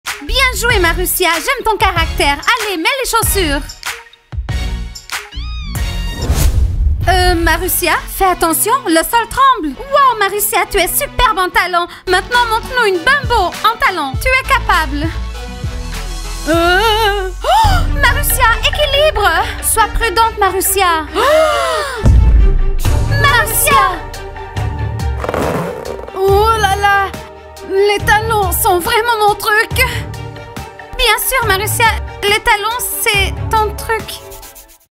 Défilé de mode chez Diana!
Défilé de mode chez Diana! Des extraits de moments amusants avec Diana!
Activez TOUTES les notifications push 🔔🔔🔔